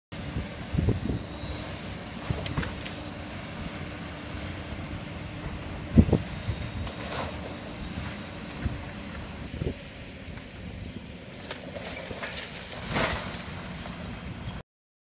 Washington County's Annex II being razed
First the Washington County Senior Center and now Annex II meeting its demise.